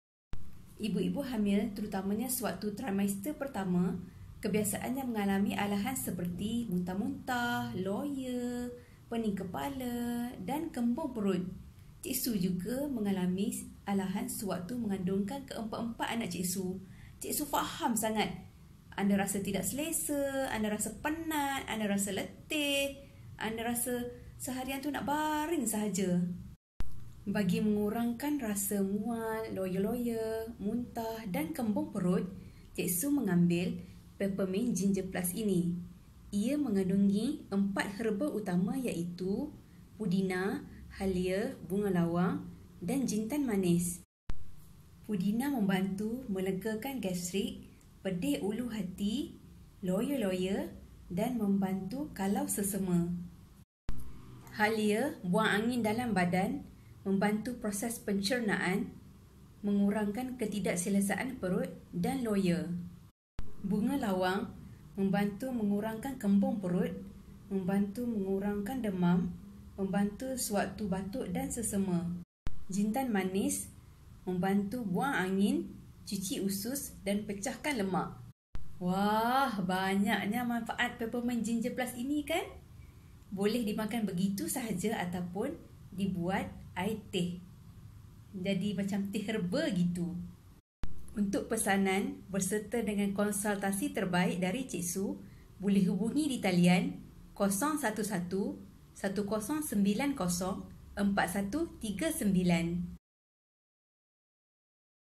Cara Mengatasi Loya Dan Muntah Sewaktu Hamil
Sewaktu hamil terutamanya di dalam trimester pertama, kebanyakan ibu hamil mengalami alahan seperti loya, mual, muntah, dan kembung perut. Bagaimana mengurangkan alahan ini? Jemput tonton video ini.